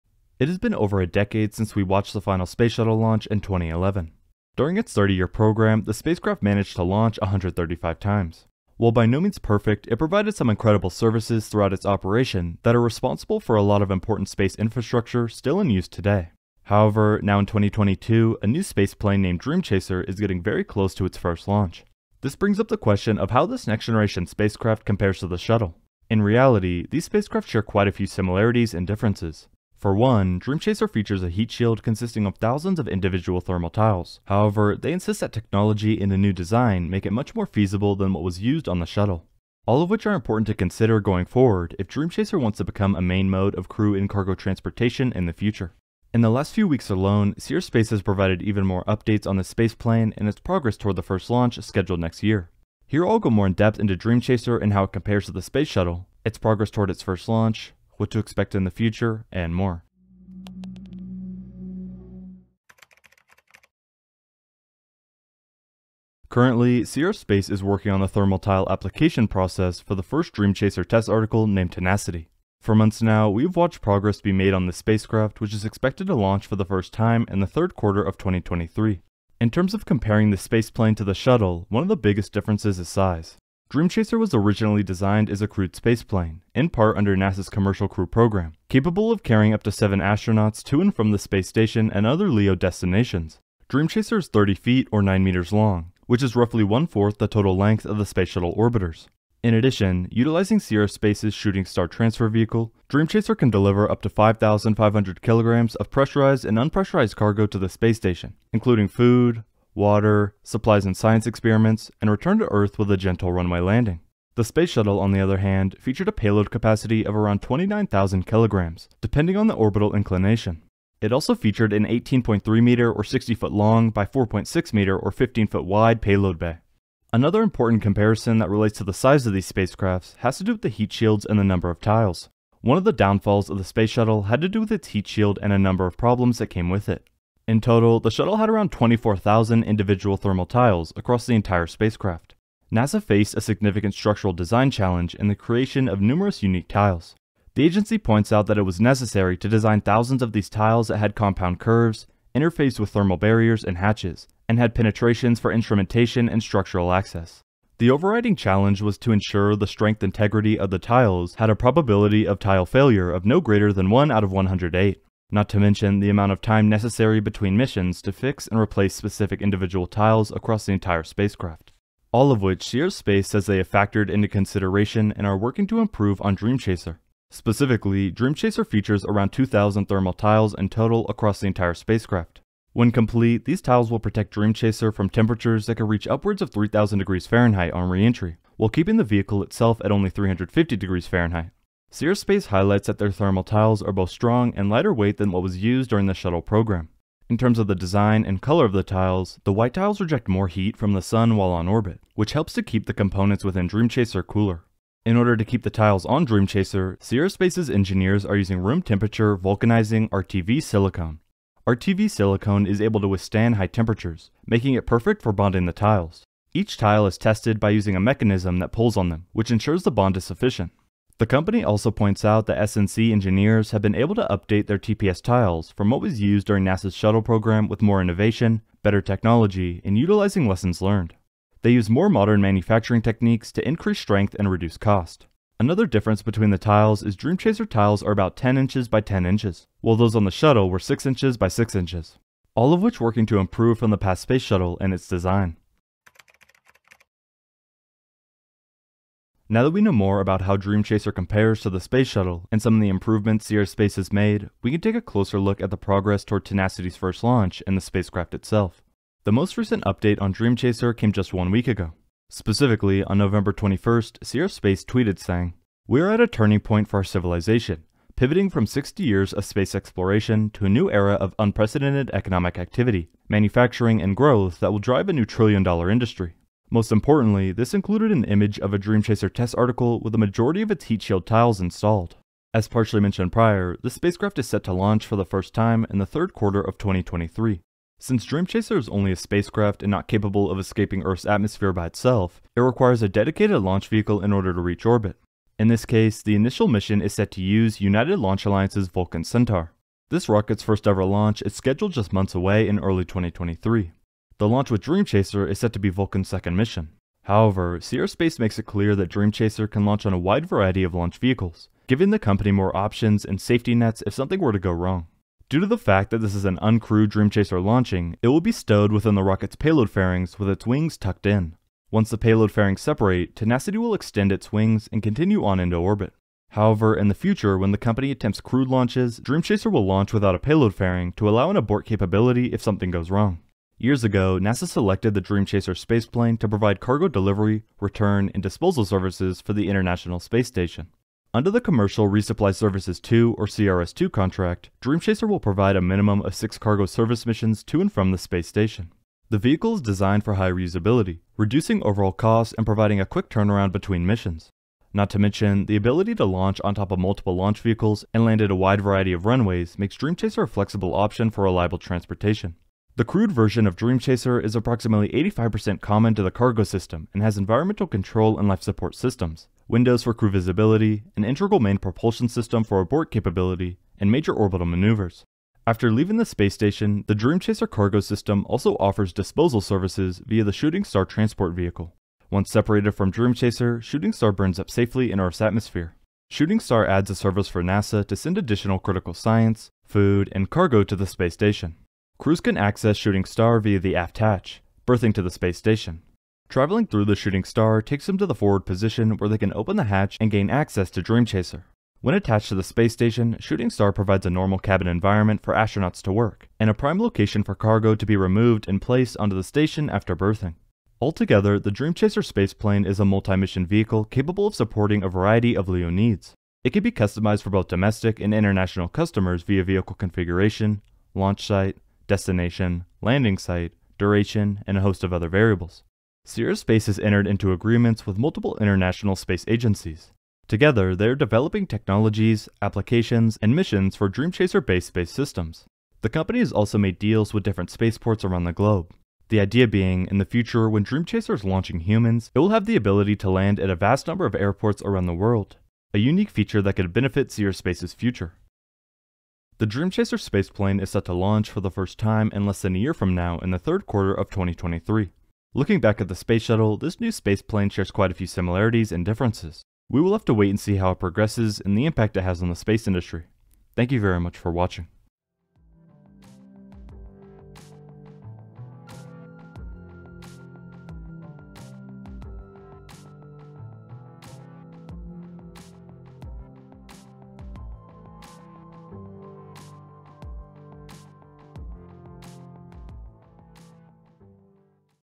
Is Dream Chaser A New & Improved Space Shuttle?
It has been over a decade since we watched the final Space Shuttle launch in 2011. During its 30-year program, this spacecraft managed to launch 135 times. While by no means perfect, it provided some incredible services throughout its operation that are responsible for a lot of important space infrastructure still in use today. However, now in 2022, a new spaceplane named Dream Chaser is getting very close to its first launch.

This brings up the question of how this next-generation spacecraft compares to the shuttle. In reality, these spacecraft share quite a few similarities and differences. For one, Dream Chaser features a heat shield consisting of thousands of individual thermal tiles, however, they insist that technology and a new design make it much more feasible than what was used on the shuttle. All of which are important to consider going forward if Dream Chaser wants to become a main mode of crew and cargo transportation in the future.

In the last few weeks alone Sierra Space has provided even more updates on this spaceplane and its progress toward the first launch scheduled next year in 2023. Here I will go more in-depth into Dream Chaser and how it compares to the Space Shuttle, its progress toward its first launch, what to expect in the future, and more.

Chapters:
0:00 - Intro
1:02 - Spacecraft Comparison
4:05 - Upcoming First Launch